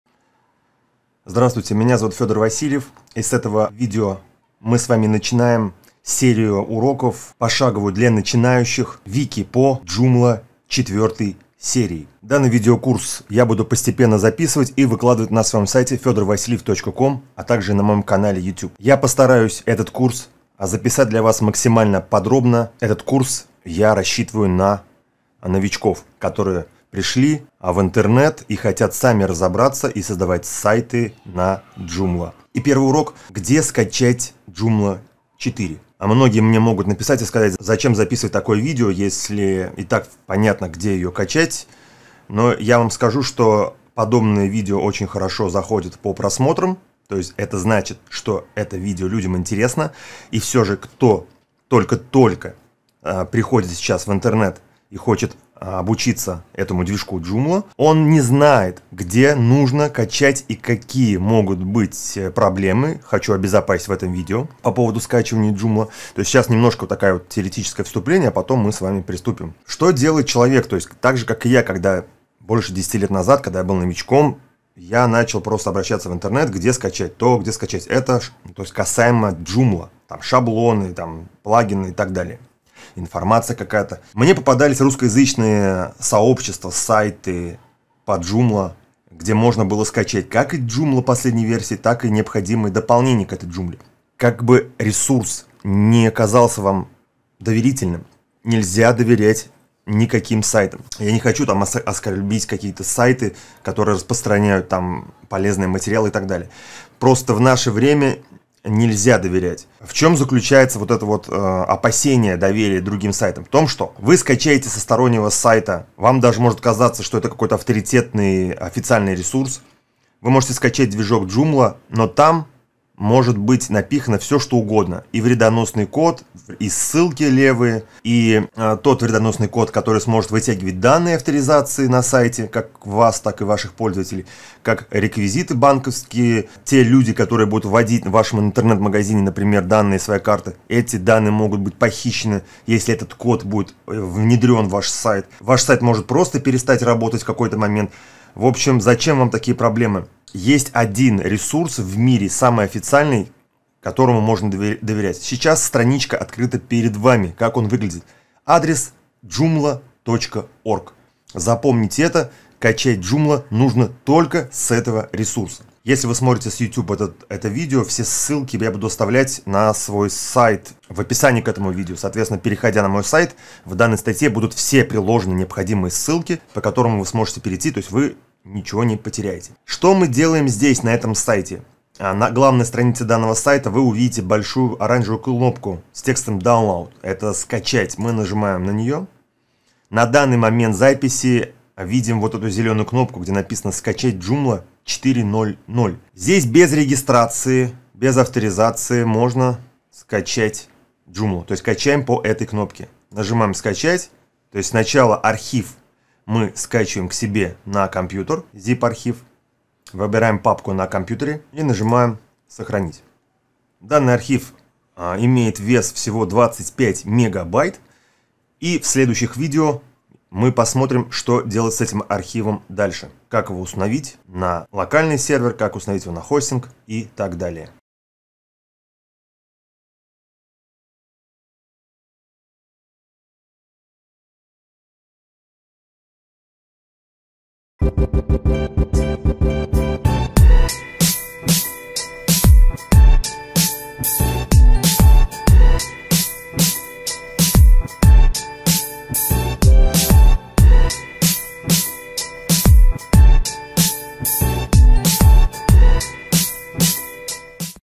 Где скачать Joomla 4
Очень важно знать где нужно скачивать актуальную версию движка Joomla 4, так как скачивая движок со сторонних сайтов вы подвергаете себя опасности словить вирус, или вредоносные ссылки, вшитые злоумышленниками в движок Joomla 4.